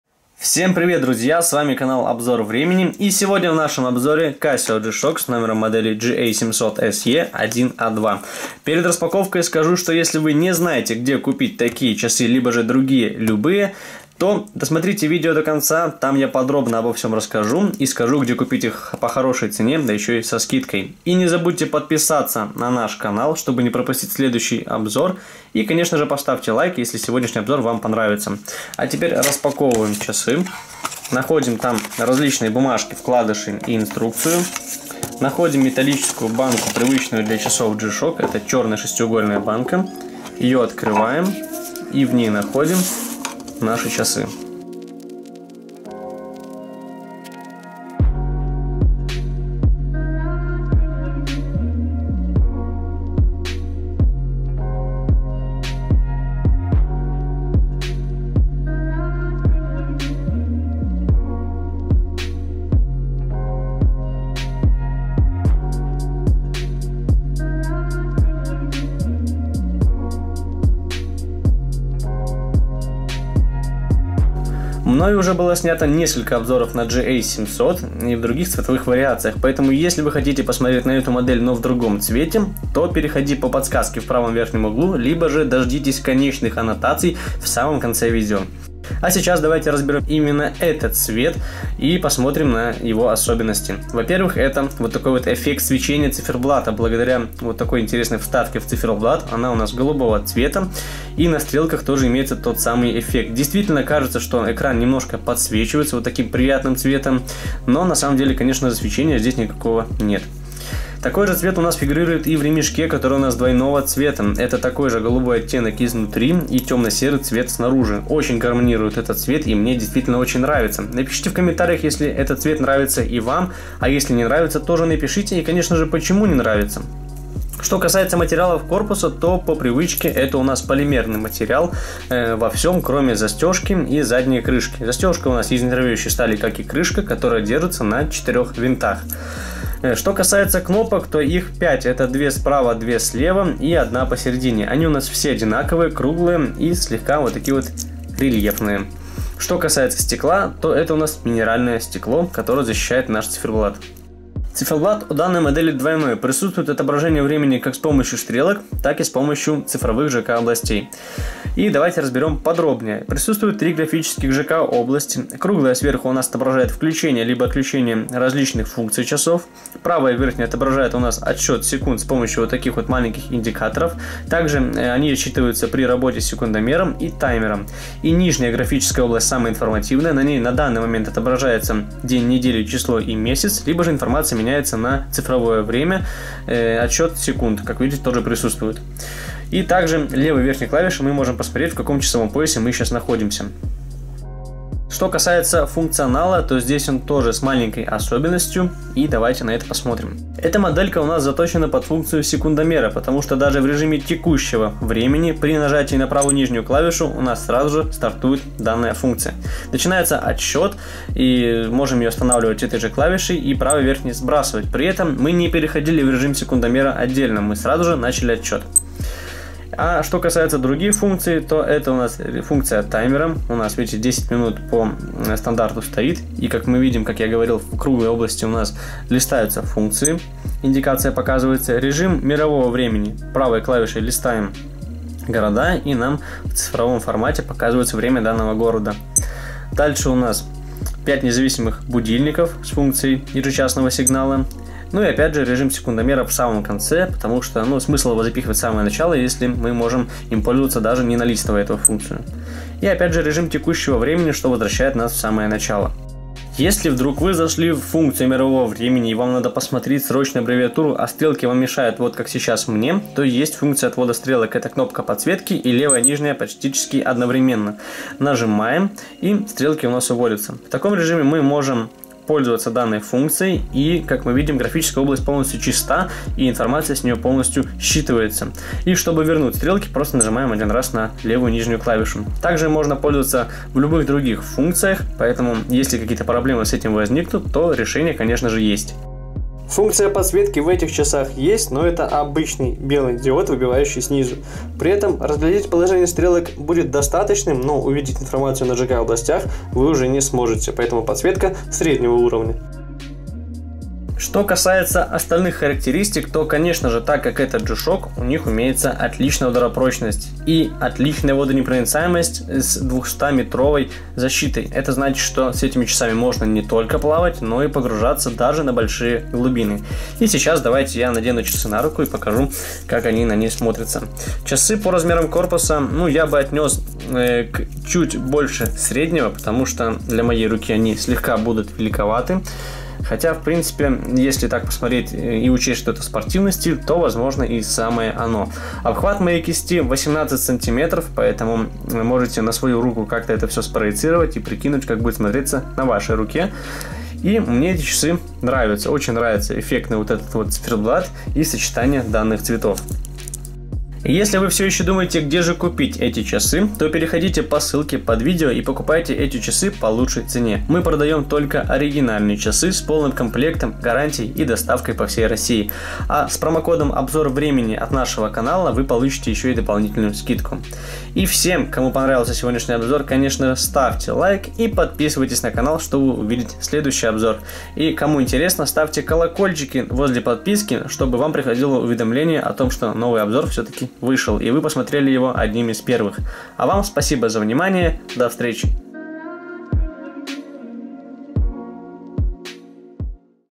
Обзор CASIO G-SHOCK GA-700SE-1A2 | Где купить со скидкой
GA-700SE — новая неоновая серия часов Casio G-Shock.Модель GA-700SE-1A2 является наиболее ярким представителем коллекции Слоистый неон

«Слоистый неон» — так называет производитель обновленную коллекцию. Если присмотреться [к не очень крупным фото] можно заметить другой цветной слой с тыльной стороны ремешка. Видимо, это и есть расшифровка названия. Цветная задняя часть ремешка смотрится оригинально и мы довольно часто встречаем такое решение у Casio. А в целом, это все та же модель GA-700 обзор и настройку функций которой Вы можете найти у нас на канале.